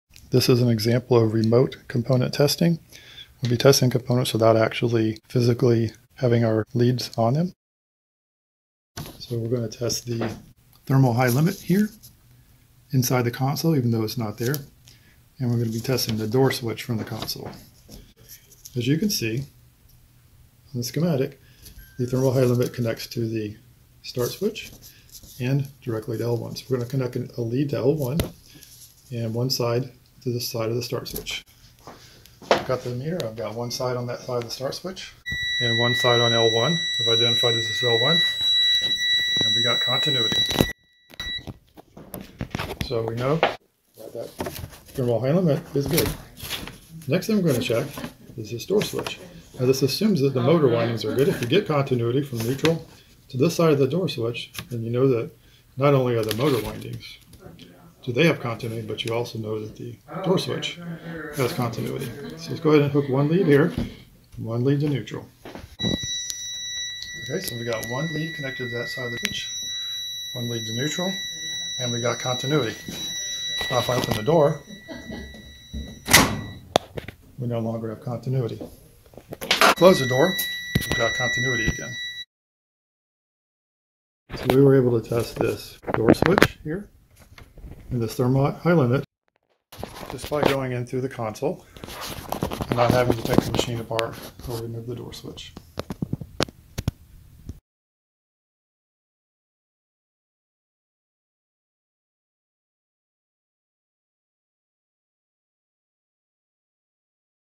#38 - Schematic Tracing using Common Electrical Points
Test components remotely to save disassembly time. Test the thermal hi-limit and start switch on a GE dryer from the console. Link to the Klein Multimeter in this video